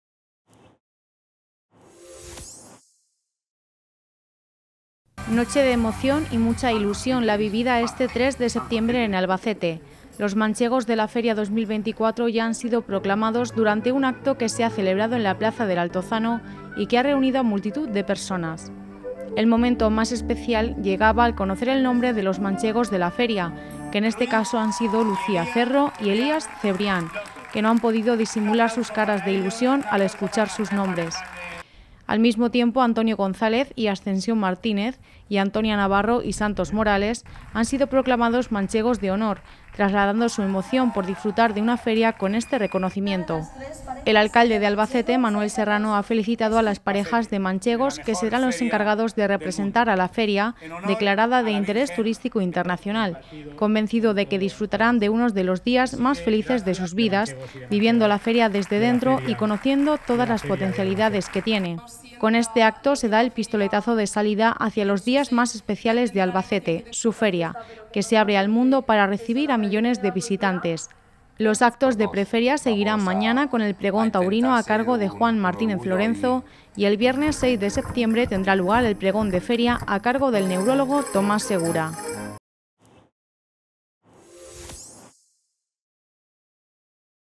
Lucía Cerro y Elías Cebrián, proclamados Manchegos de la Feria de Albacete 2024
- La proclamación de los Manchegos marca la cuenta atrás para el inicio de la Feria de Albacete 2024
El acto se ha  celebrado en la Plaza del Altozano reuniendo a multitud de albaceteños y albaceteñas en torno a uno de los momentos más emocionantes de la Feria de Albacete.